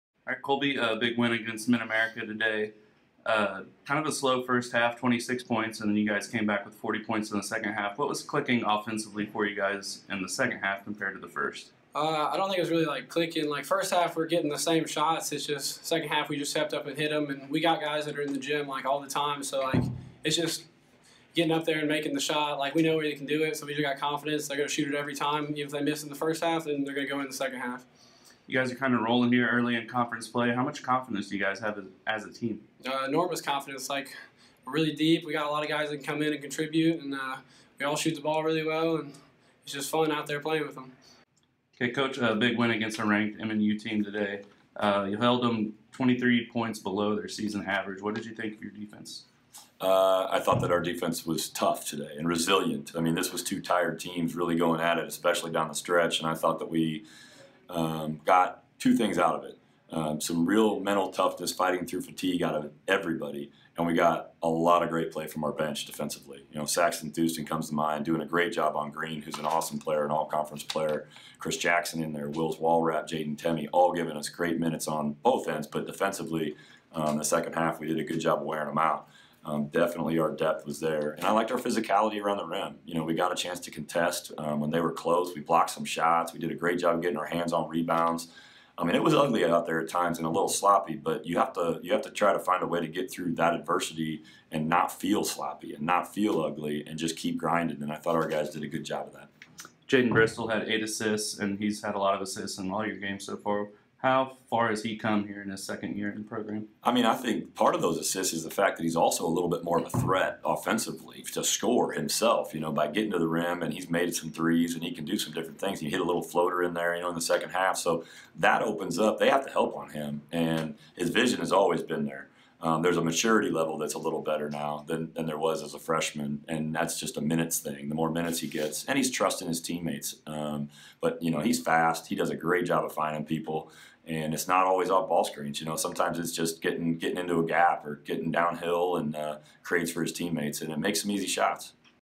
Men's Basketball Postgame vs MNU
Hear from Junior Colby Nickels and Head Coach Ryan Moody after their 66-55 win over MNU.